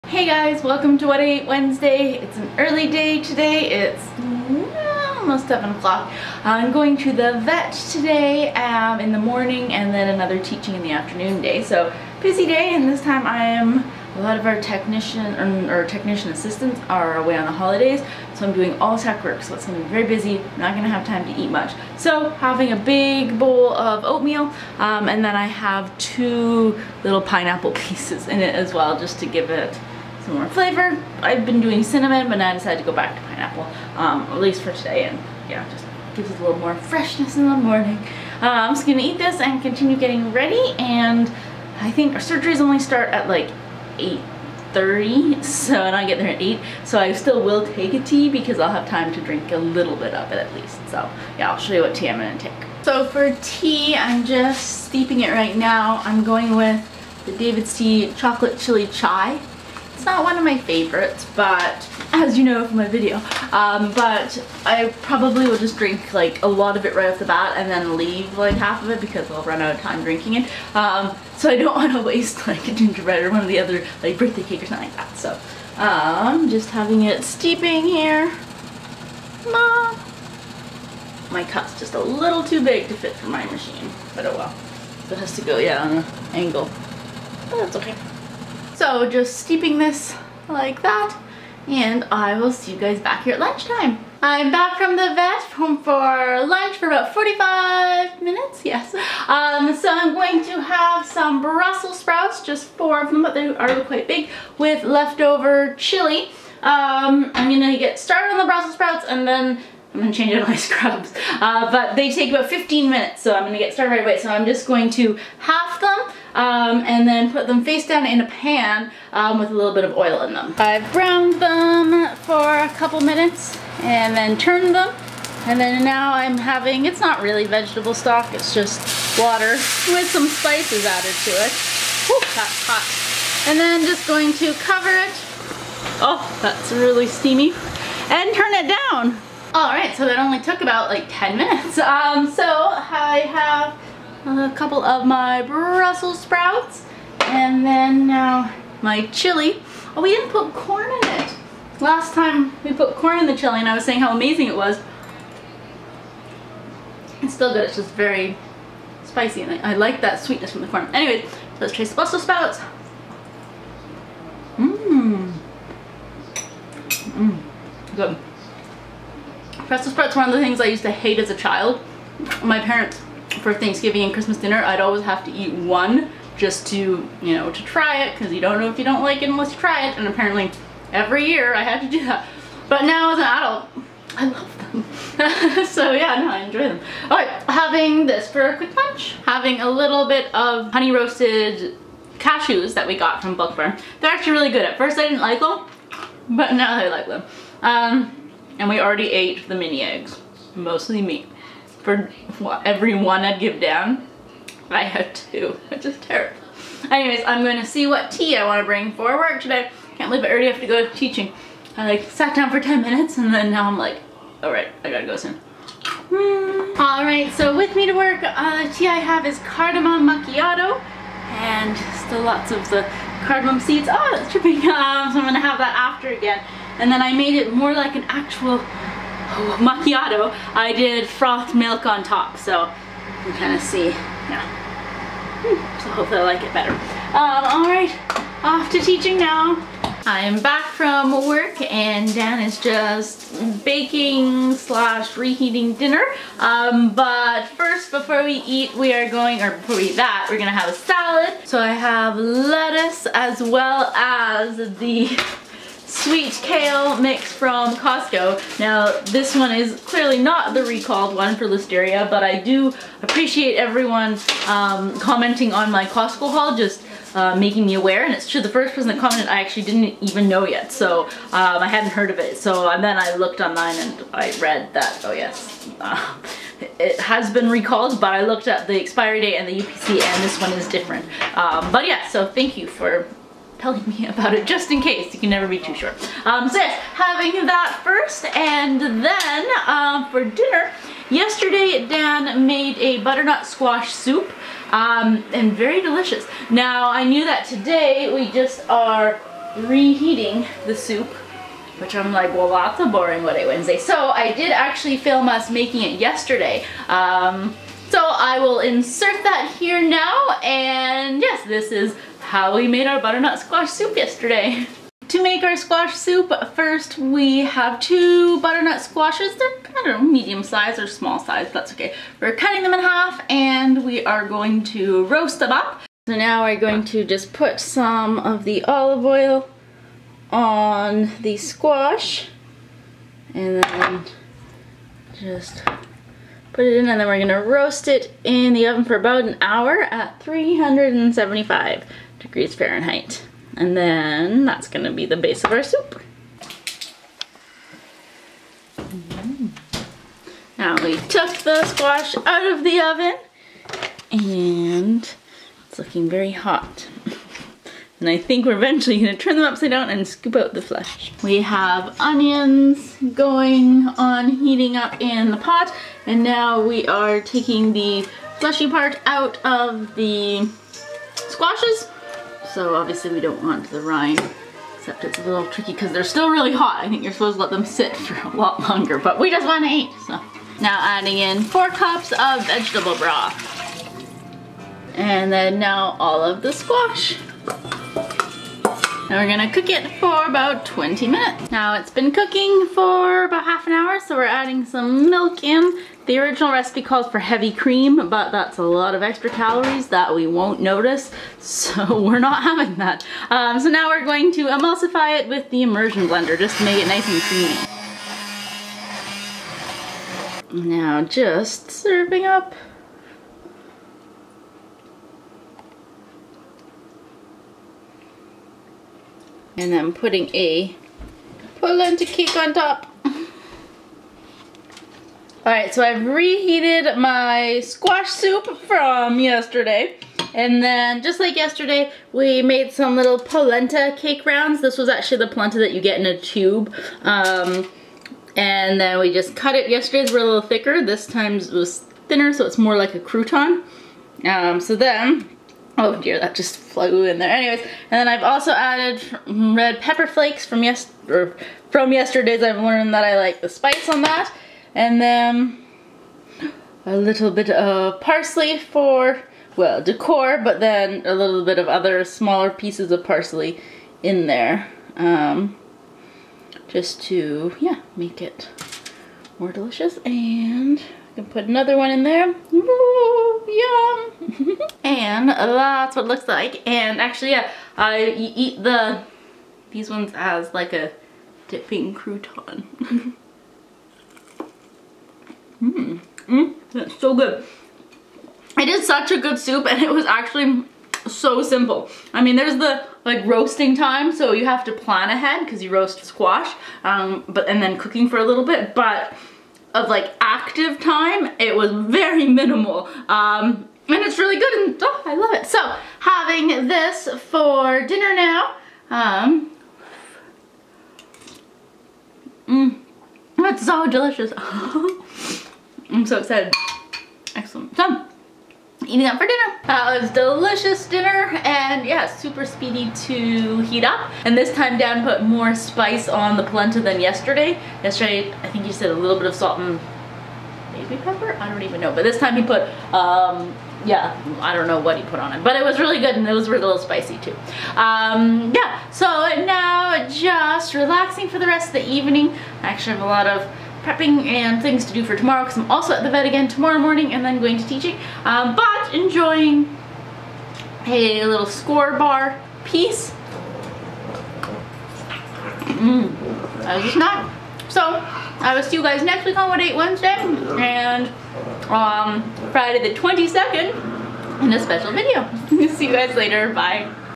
What I Ate: Easy Squash Soup with Polenta Croutons | Feb 20
What I Ate Wednesday February 20 - Easy butternut squash soup with polenta croutons! I was surprised how easy it was to make the soup but it tastes amazing!

Send me a card or postcard:

Christy Len
Victoria, BC
V8T5G2
Canada